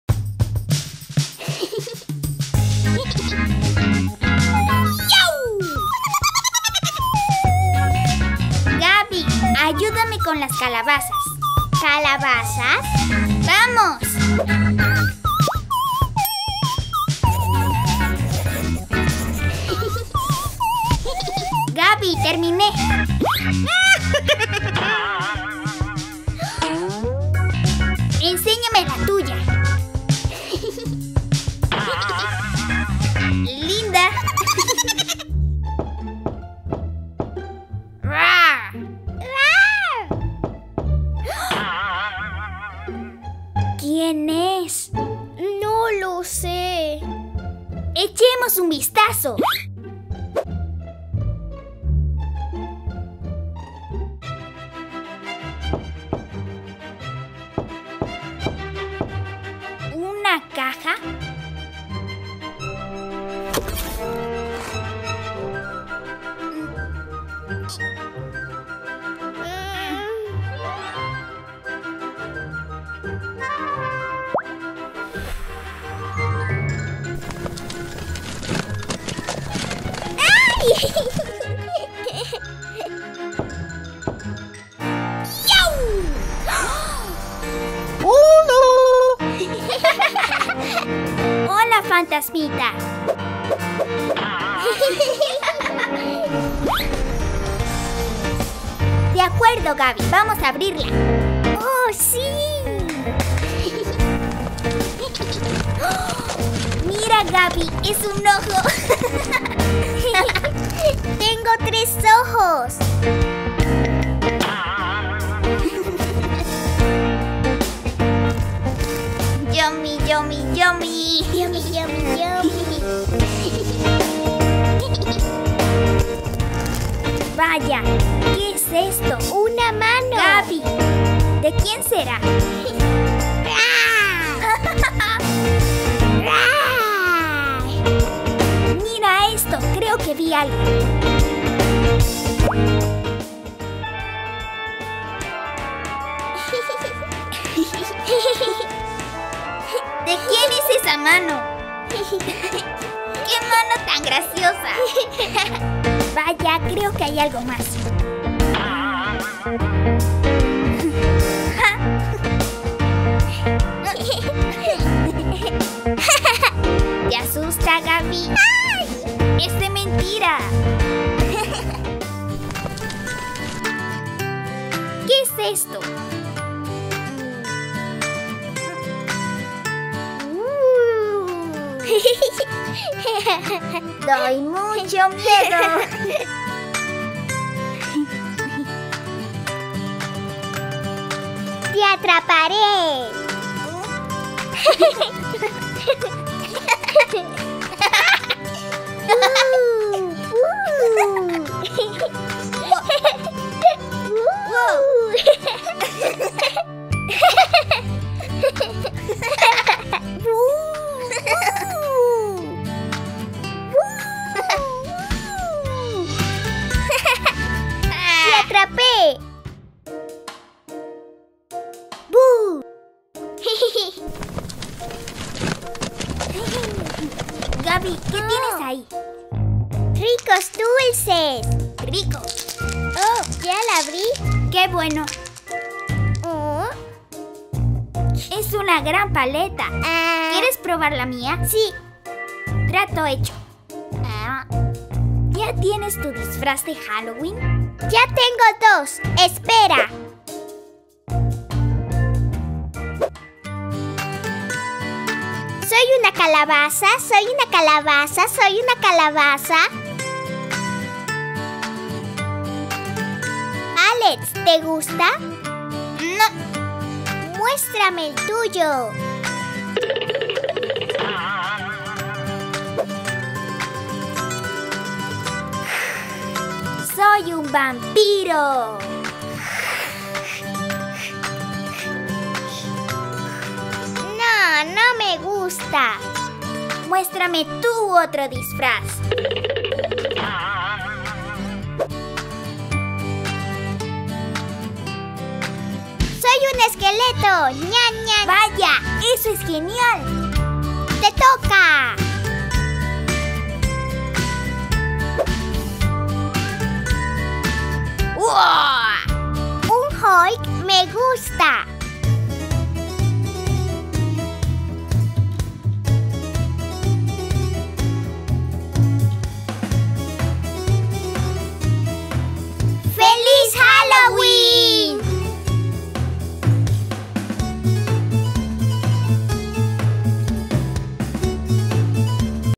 Gaby y Alex eligen un disfraz de Halloween y juegan con juguetes de Halloween
Espero que disfruten esto!